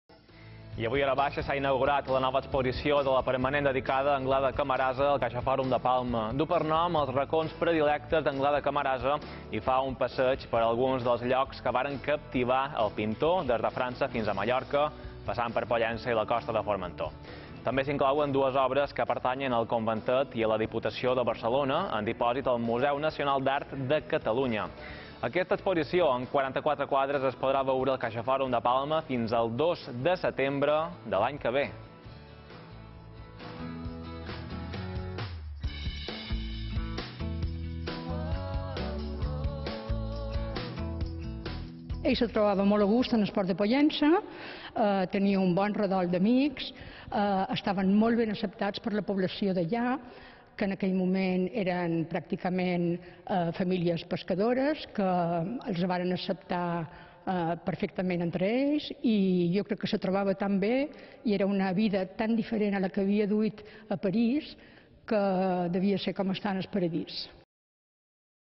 Inaugurada la nova exposició dedicada a Anglada-Camarasa al Caixaforum
Aquest horabaixa s’ha inaugurat la nova exposició de la permanent dedicada a Anglada-Camarasa al Caixaforum de Palma. Du per nom “Els racons predilectes d’Anglada-Camarasa” i fa un passeig per alguns dels llocs que varen captivar el pintor, des de França fins a Mallorca, passant per Pollença i la costa de Formentor. També s’inclouen dues obres que pertanyen al conventet i a la Diputació de Barcelona, en dipòsit al Museu Nacional d’Art de Catalunya. Aquesta exposició amb 44 quadres es podrà veure al CaixaForum de Palma fins al dos de setembre de l’any que ve.